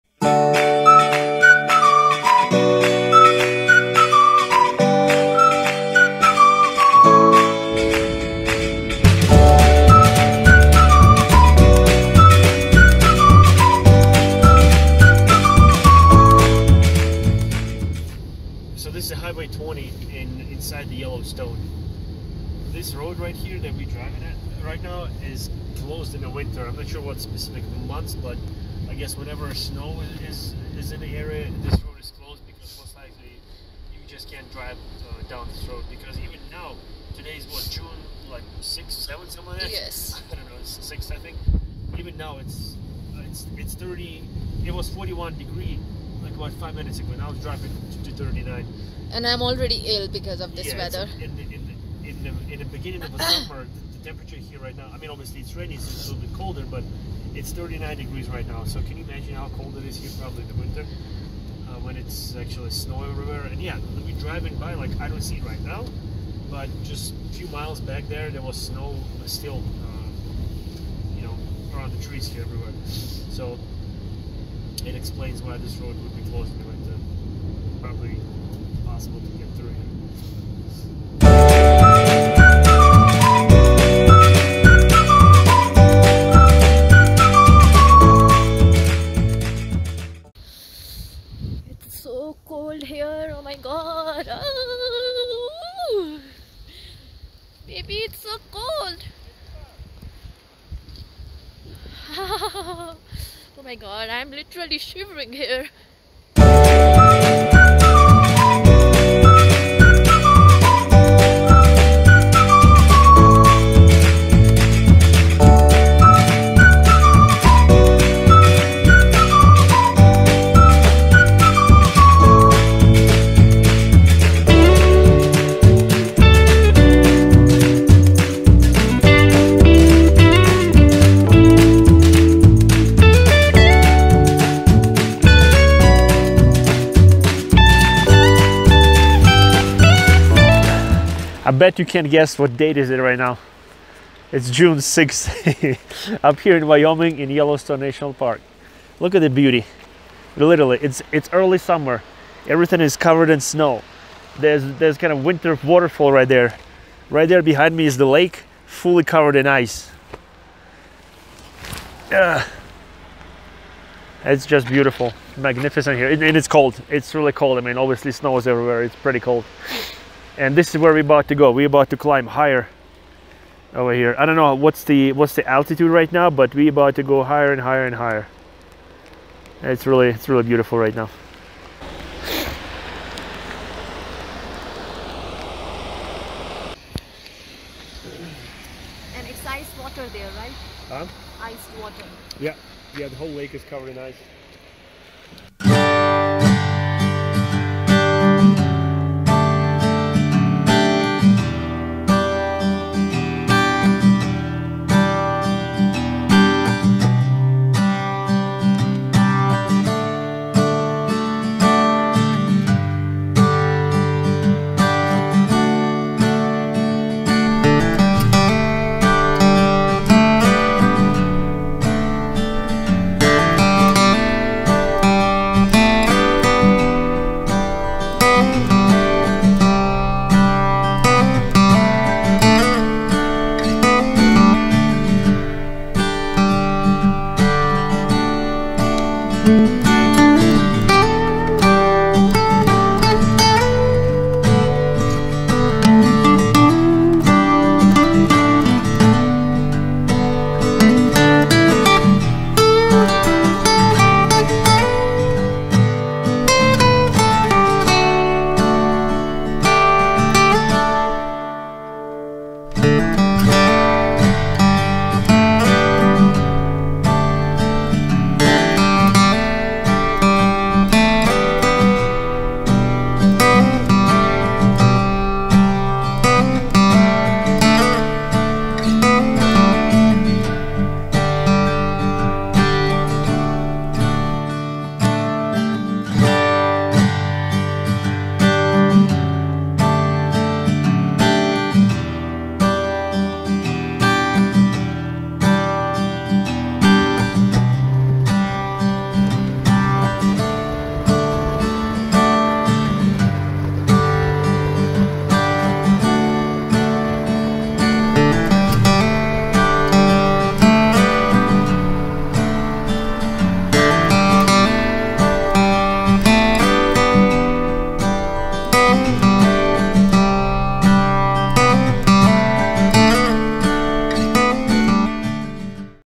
Winter in June - Breathtaking beauty of Wyoming
Nothing compares to a Wyoming road trip. The Rocky Mountains and the High Plains meet right here in Wyoming, resulting in stunning views in every direction. Wyoming has over 20 scenic byways that twist, turn and climb through some of the state’s most breathtaking landscapes. Plan a drive along these Wyoming roads to immerse yourself in wild beauty and western history.